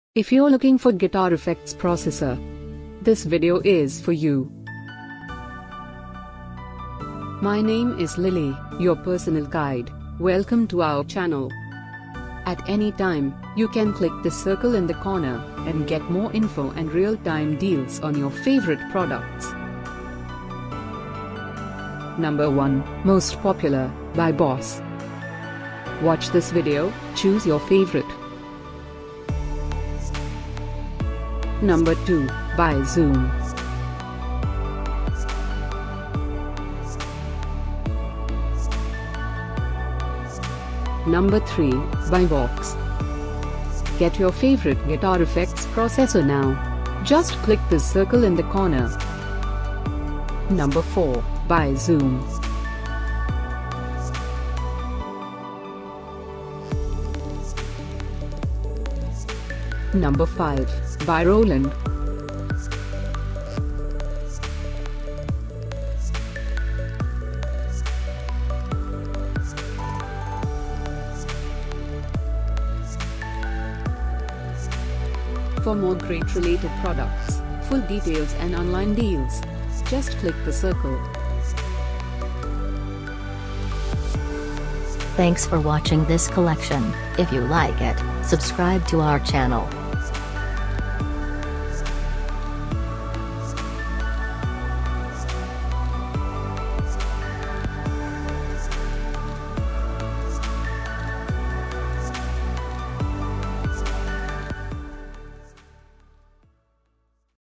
Top 10 Guitar Effects Processor [2018]: Boss Gt-1 Guitar Effects Processor
Top 10 Guitar Effects Processor [2018]

Boss Gt-1 Guitar Effects Processor by BOSS

Zoom G1Xon Guitar Effects Pedal (Black) by Zoom

VOX 2G SL2G Stomplab Guitar Processor by Vox

Zoom G1oN Guitar Effects Pedal (Black) by Zoom

Roland ME-80 Guitar Multiple Effects, Black by Roland

Zoom G5n Multi-Effects Guitar Processor by Zoom

Boss GT-001 Guitar Effects Processor by BOSS